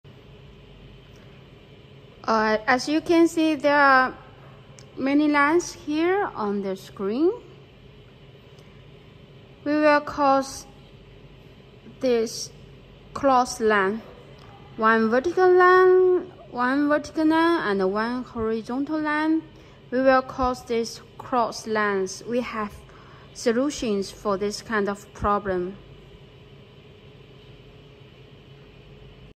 Cross line on panels can be repaired by our laser machine
Cross line is a mix of bright vertical line and bright horizontal line. The problem will be the intersection point of the 2 lines.